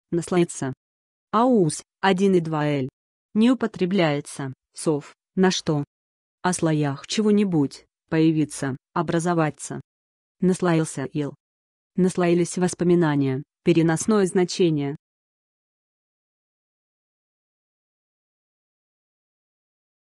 НАСЛОИТЬСЯ - что это такое? значение и описание
Хочешь знать что значит НАСЛОИТЬСЯ? тогда слушай до конца. n n (-оюсь, -оишься, 1 и 2 л. не употр.), -оится; сов., на что. О слоях чего-н.: появиться, образоваться. Наслоился ил. Наслоились воспоминания (перен.). || несов. наслаиваться (-аюсь, -аешься, 1 и 2 л. не употр.), -ается. || сущ. наслоение, -я, ср. n n Больше слов